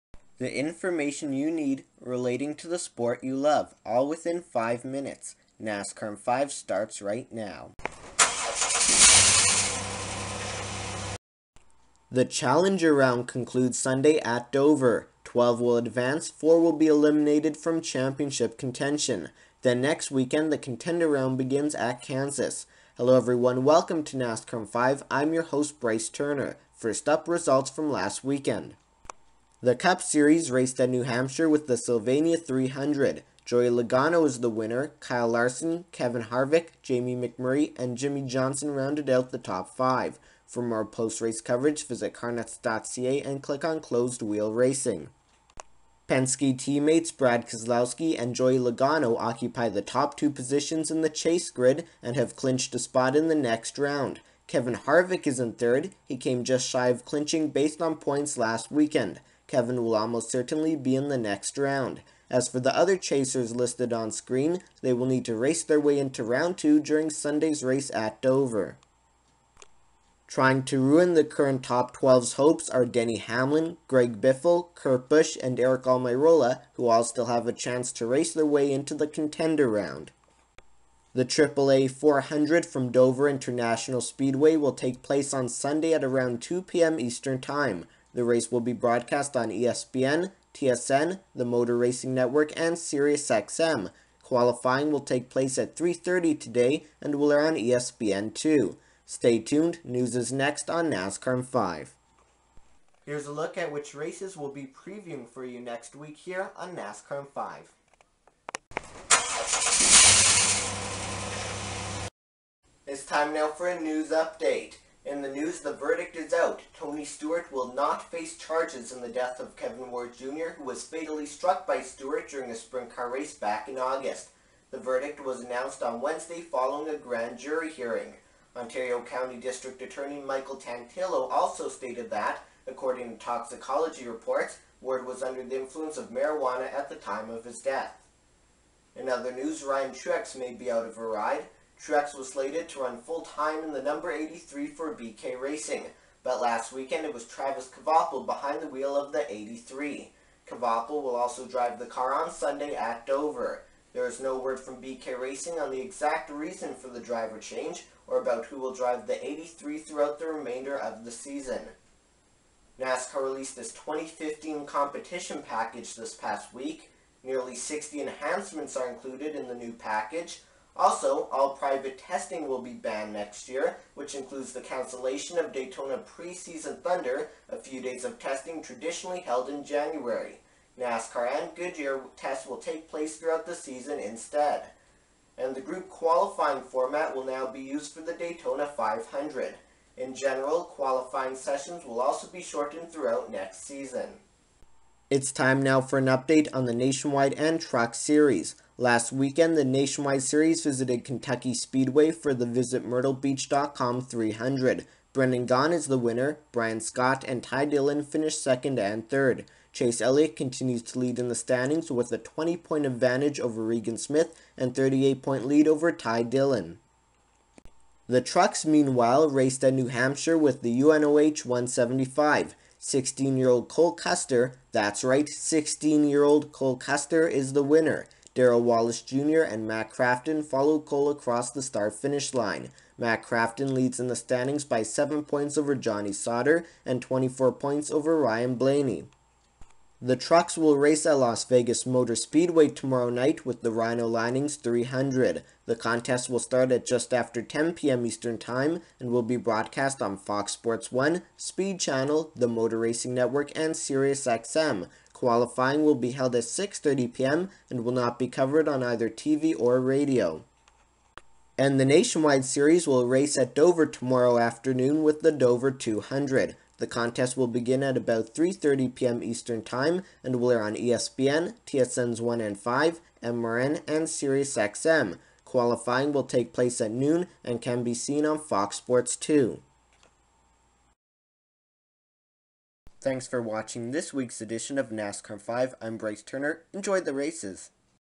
NASCAR in 5 Pre-race for Dover and Las Vegas
Pre-race coverage for the Nationwide and Cup Series races at Dover International Speedway, and Truck Series contest at Las Vegas Motor Speedway. NASCAR in 5 provides the latest news, schedules, standings, and more, all within five minutes!